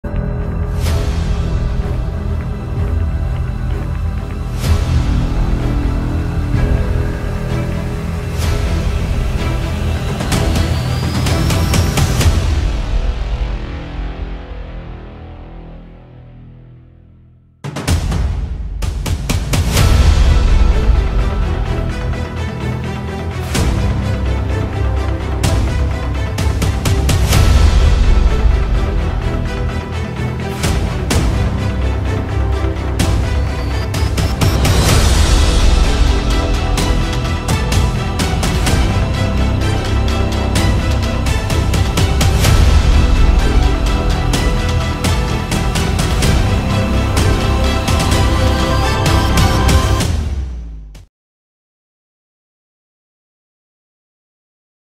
هدف اللاعب أيمن أواني في شباك حسنية أكادير في المباراة الإعدادية ليوم أمس ِ
ِِAouani's goal against HUSA in yesterday's friendly match